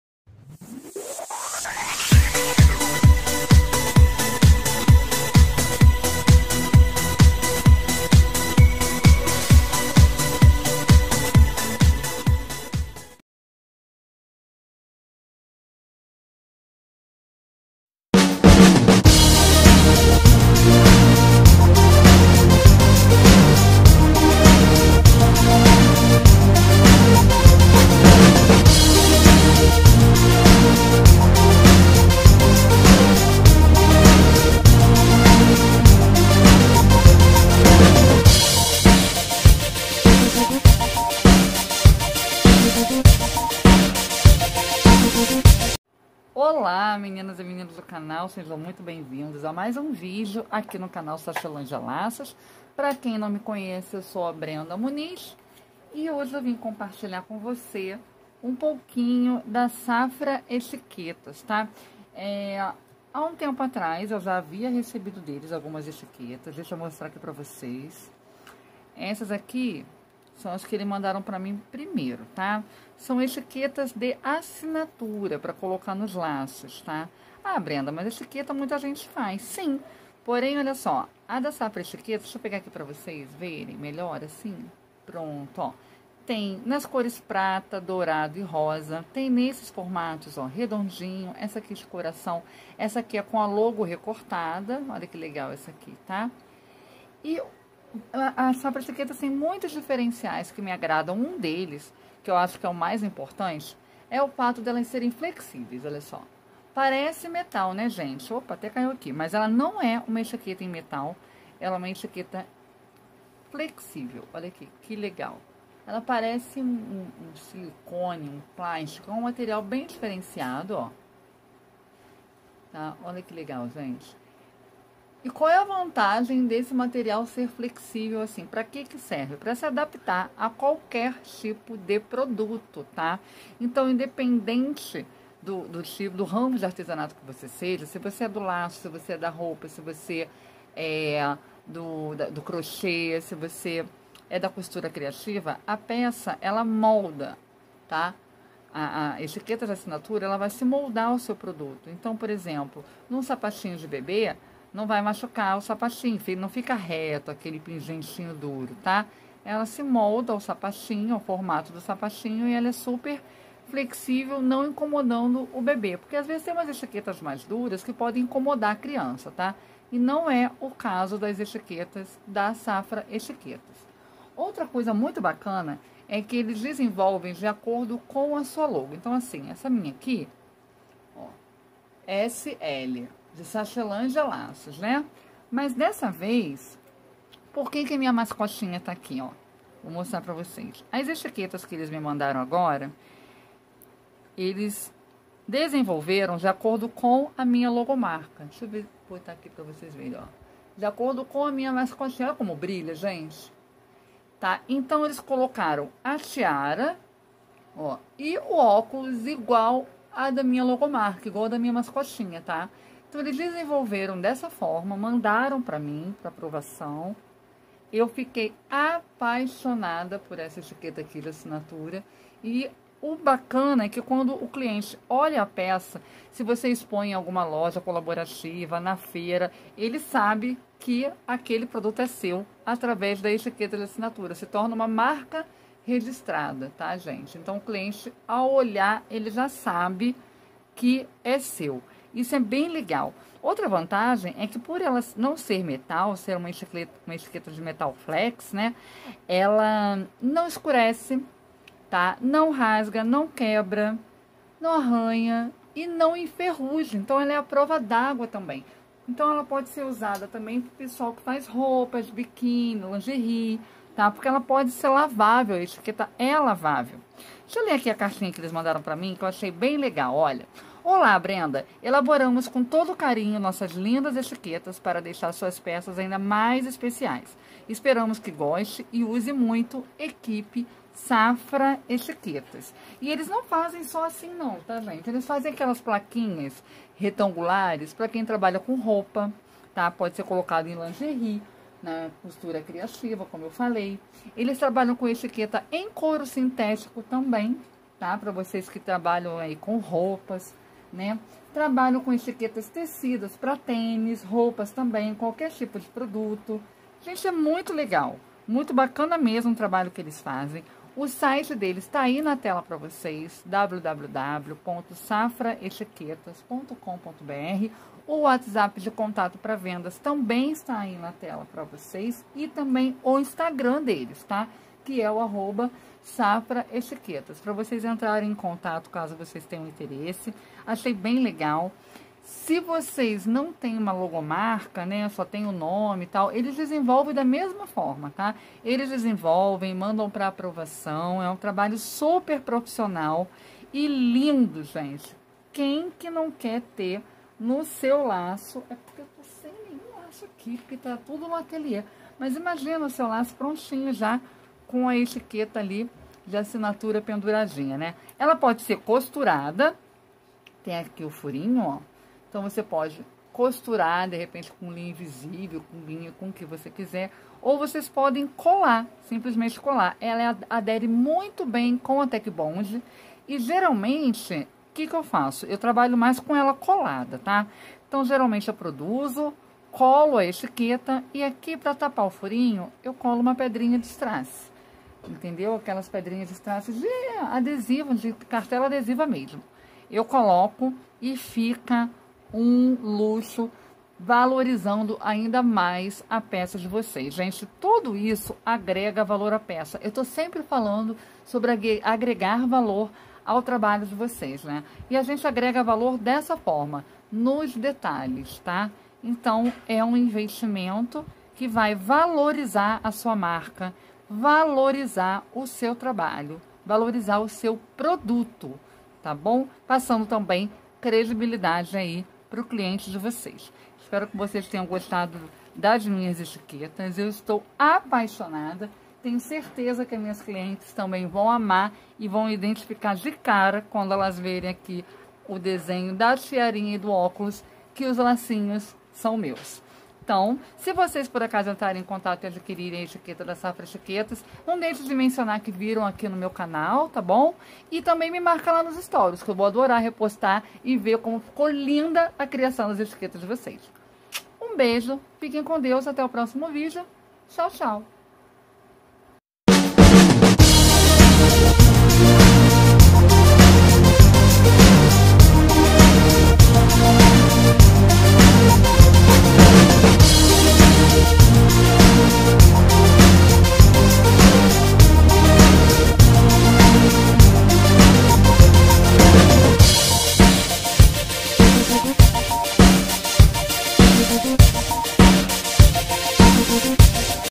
Como agregar valor ao seu produto utilizando etiquetas de assinatura - Safra Etiquetas
Safra Etiquetas